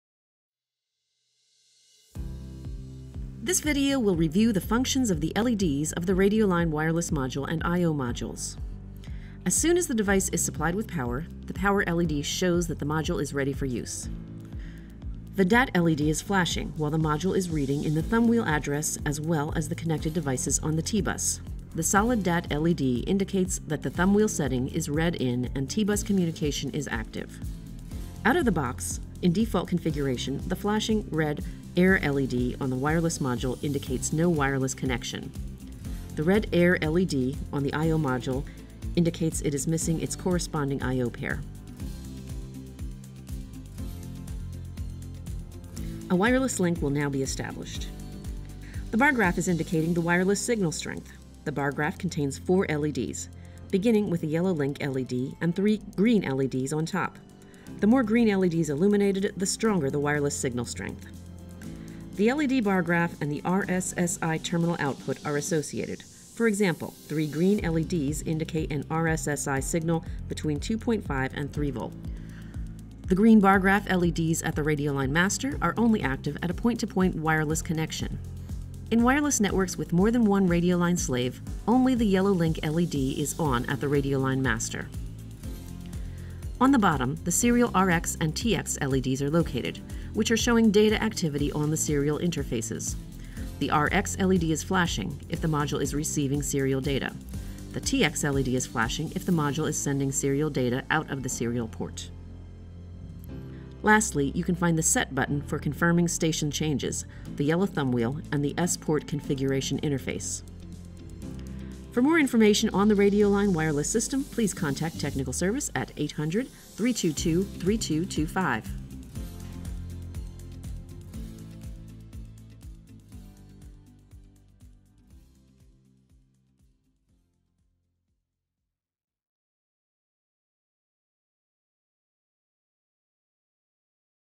Radioline – Diagnostic elements
►This video explains all diagnostic LEDs of the Radioline wireless module. Additionally many backgrounds and content from the user manual are discussed.